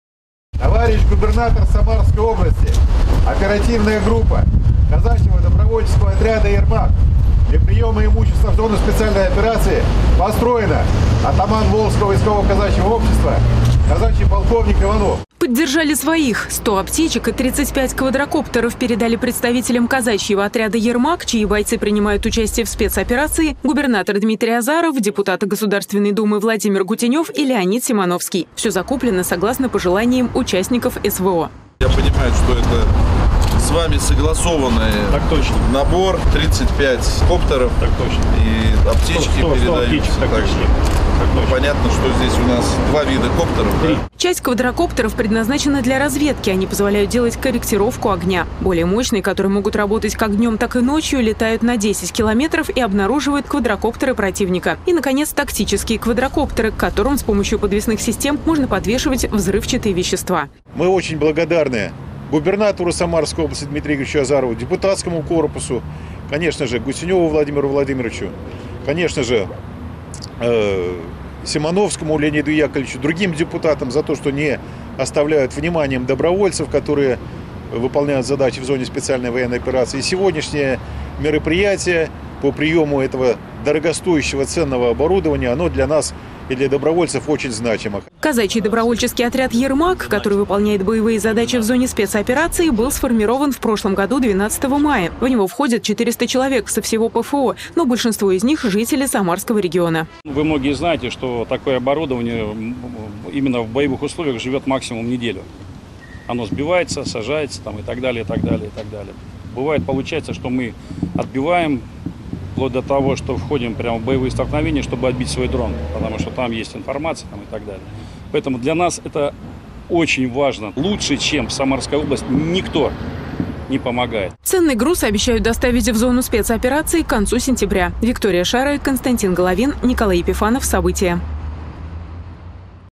Представители казачьего отряда "Ермак" передали для участников СВО 100 аптечек и 35 квадрокоптеров
100 аптечек и 35 квадракоптеров передали представителям казачьего отряда "Ермак", чьи бойцы принимают участие в спецоперации,  губернатор Дмитрий Азаров, депутаты Государственной Думы Владимир Гутенев и Леонид Симановский. Все закуплено согласно пожеланиям участников СВО. Подробности в нашем материале.